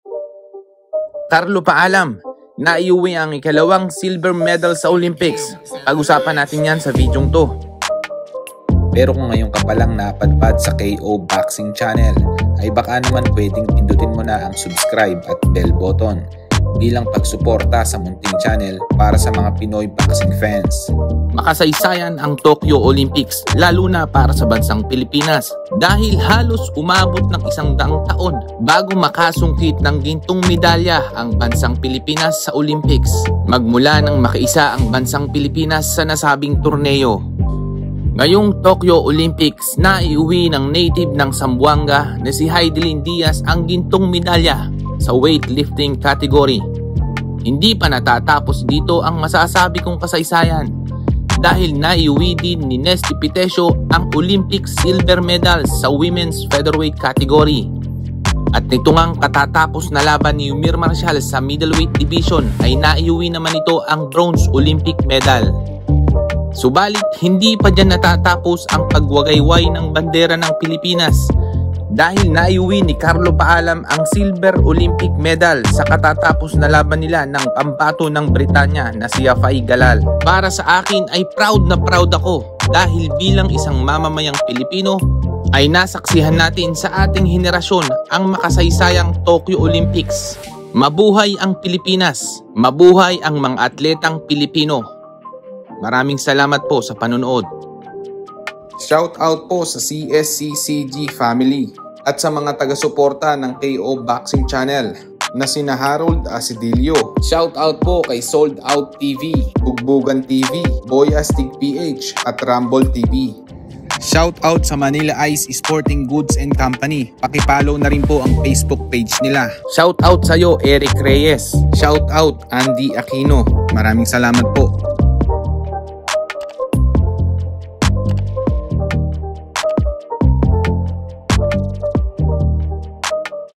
CARLO PAALAM NAIUWI ANG OLYMPIC SILVER MEDAL! [TOKYO OLYMPICS UPDATES]
CARLO PAALAM BAGS PHILIPPINE'S SECOND OLYMPIC SILVER MEDAL.

This channel and I do not claim any right over any of the graphics, images, songs used in this video.